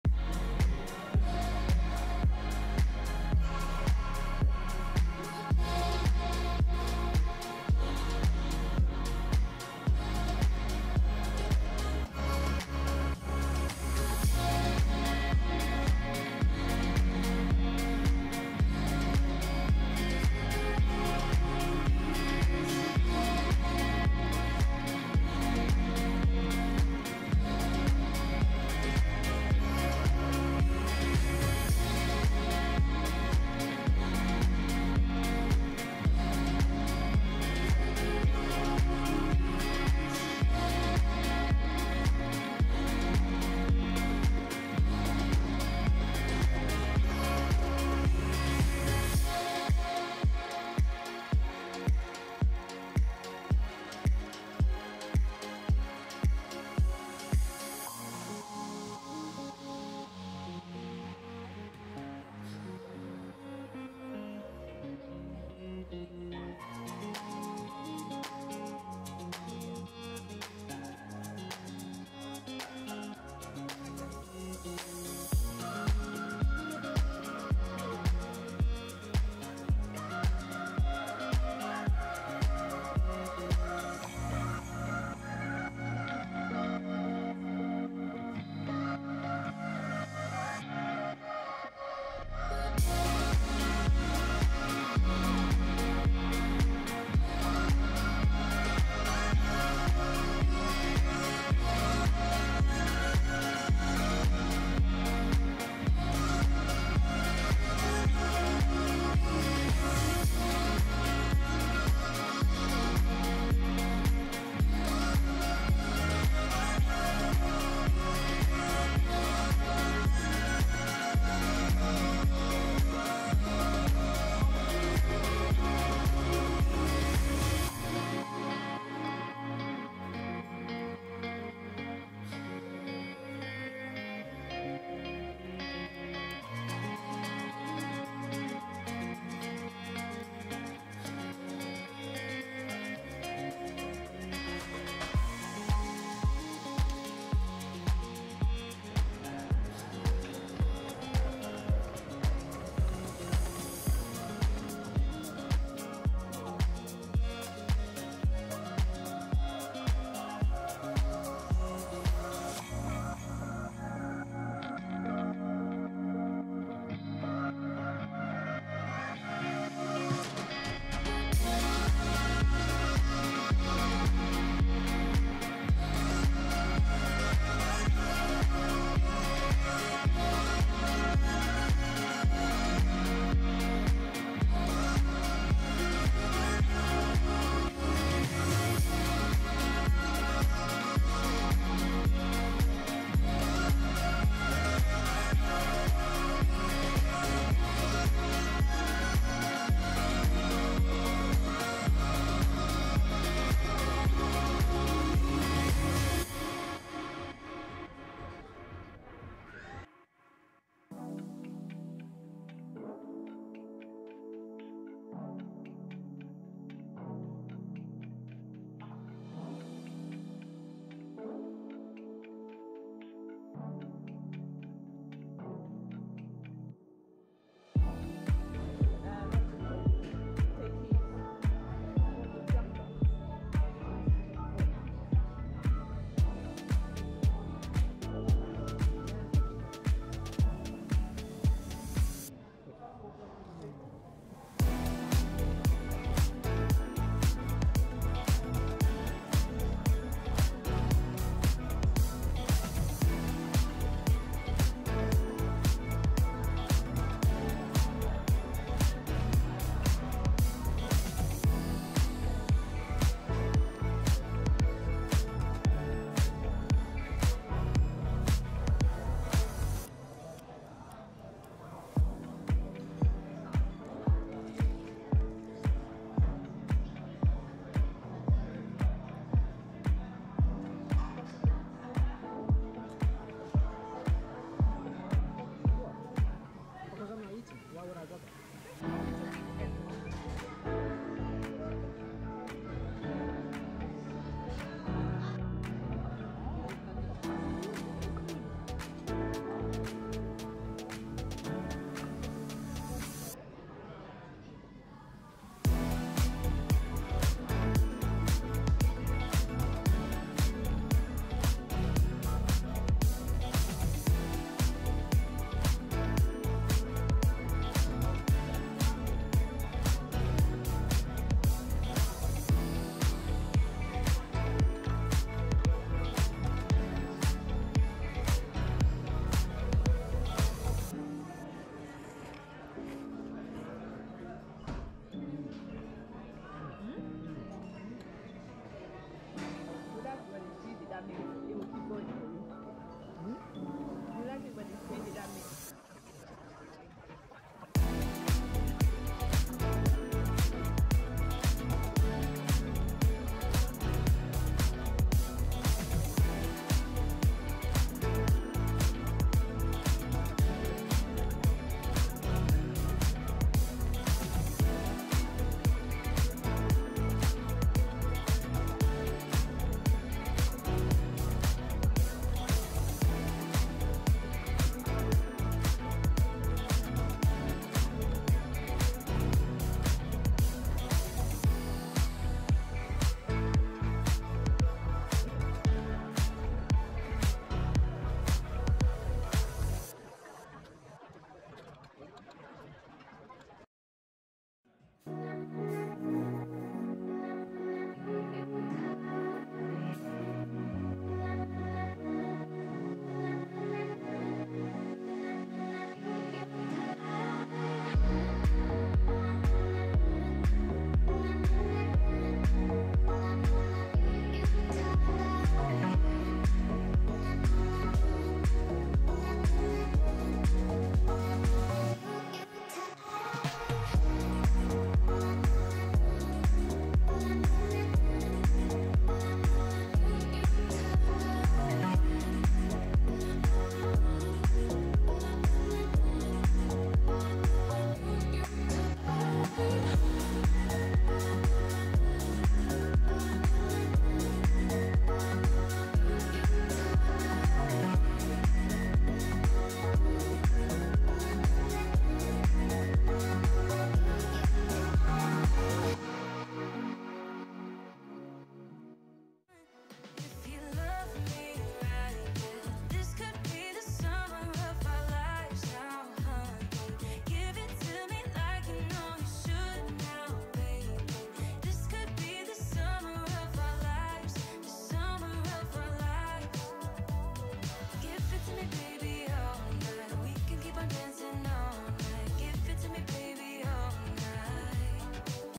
Food vlog - Nandos, Vietnamese Pho, Steak & Lobster, and more | Vlog #039
Hello!

Following my recent Mauritius food vlog, this is a compilation of some of the restaurants we visted during the past few weeks.